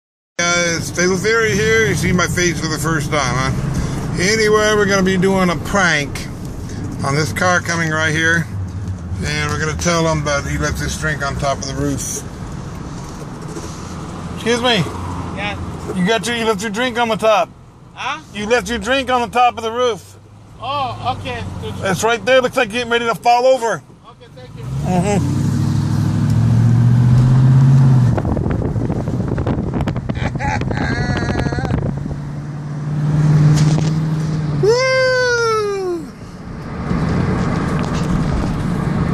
Cup on top of roof prank
Hey guys i tell people theres a cup on top of there rof prank LOL